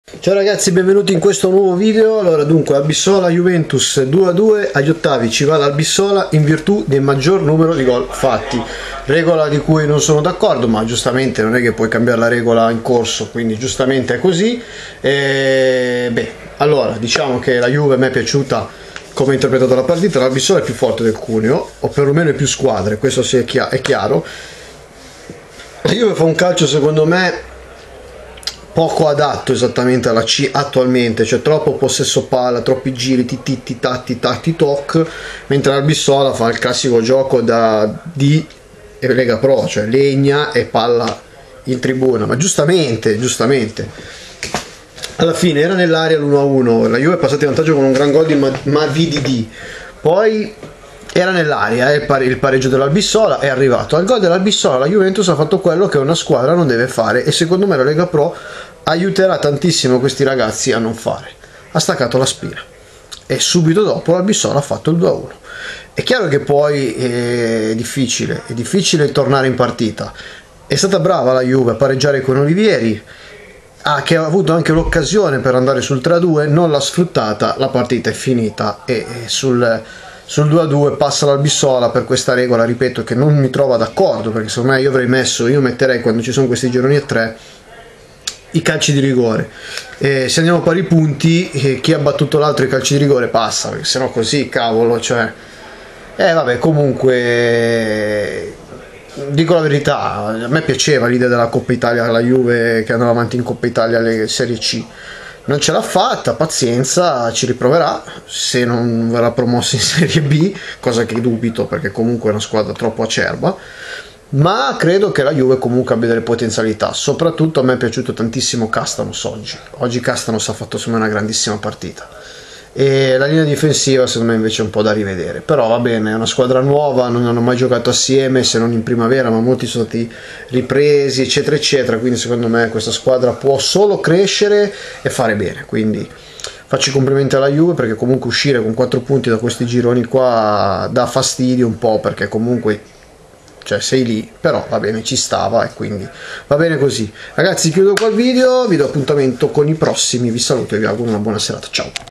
Albissola - Juventus U23 2-2
Juve che pareggia 2-2 a Chiavari con l ' Albissola ma non basta agli ottavi vanno i liguri